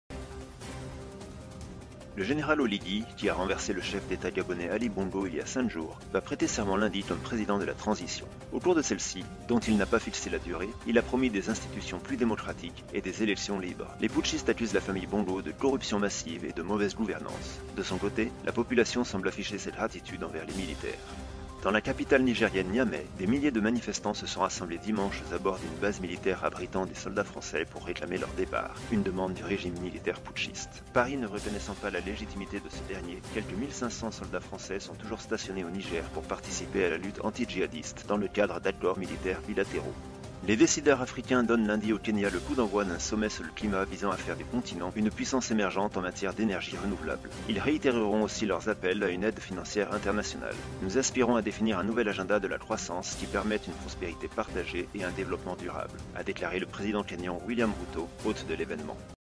🔷📍🔷 EnBrefEnAfrique le 4 septembre 2023 : Gabon, Niger, Kenya
🇬🇦 Gabon : le général Oligui va prêter serment
🇳🇪 Niger : pression populaire pour le départ des soldats français
🇰🇪 Kenya : coup d'envoi du Sommet africain pour le climat